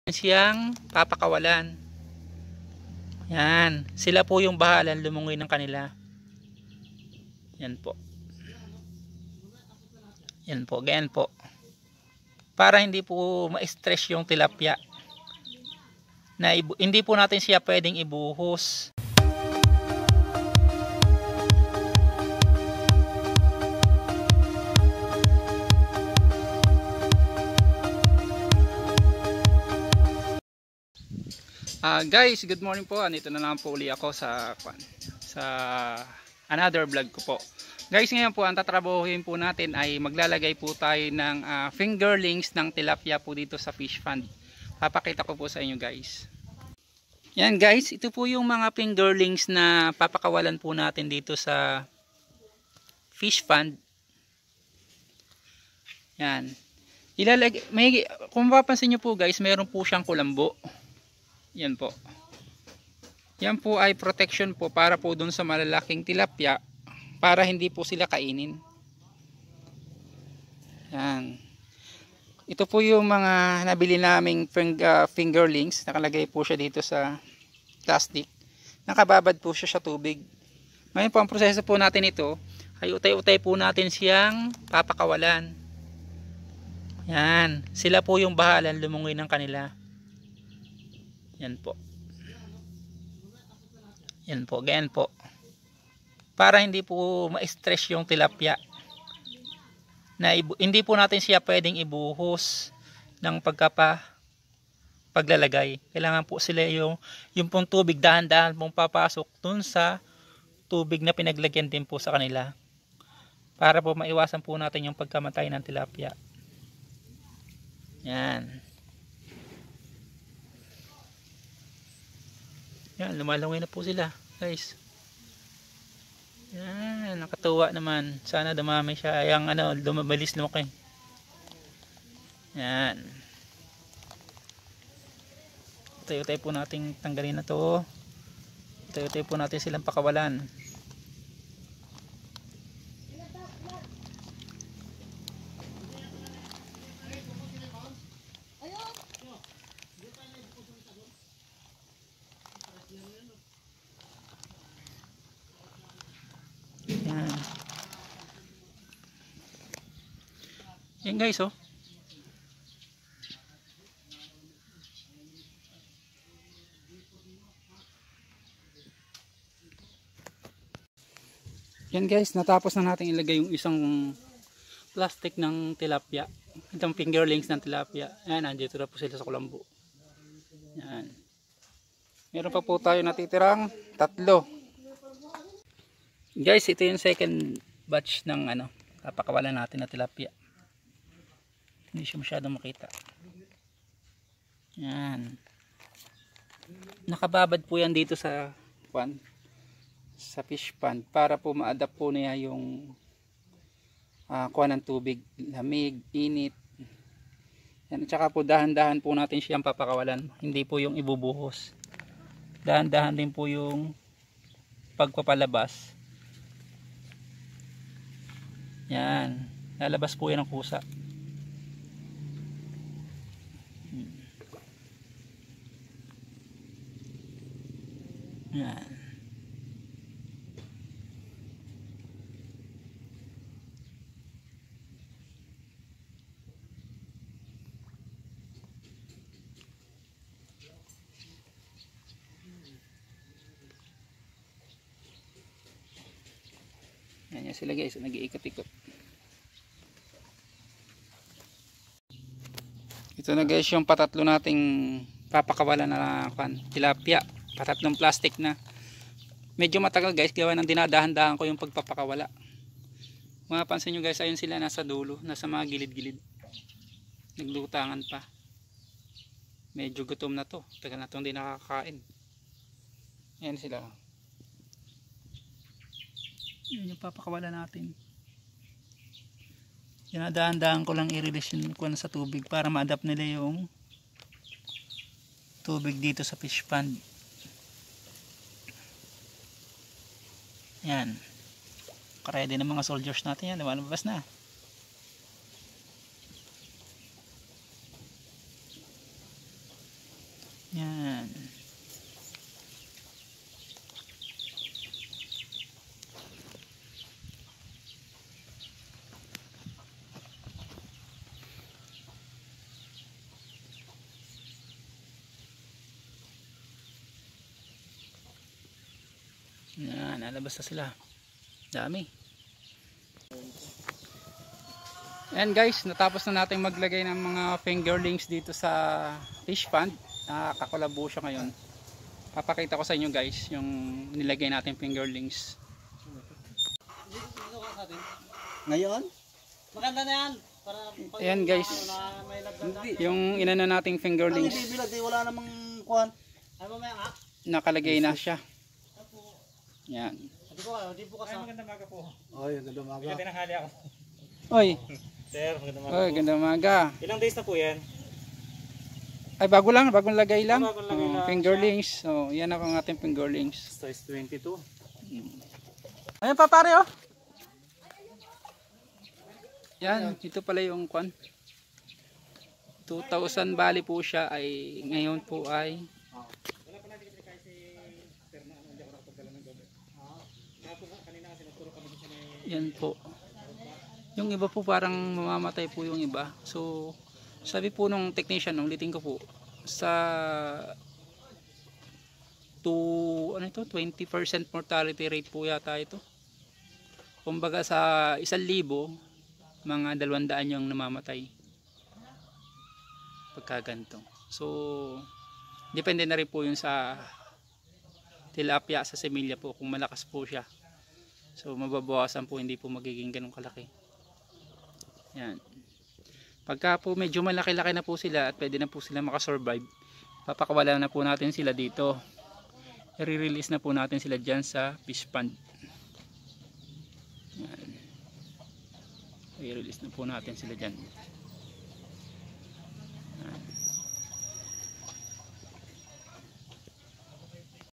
Tilapia fingerlingsPaglalagay ng tilapia fingerlings sa fishpond
Paglalagay ng bagong similya ng tilapia sa fishpond paraan para makaadap ang bagong similya sa tubig ng fishpond, kelangan nakalagay ang plastic sa fishpond para maadopt ng similya ang init at lamig ng tubig bago utay utay ng pakawalan ang mga fingerlins. hayaan silang lumangoy.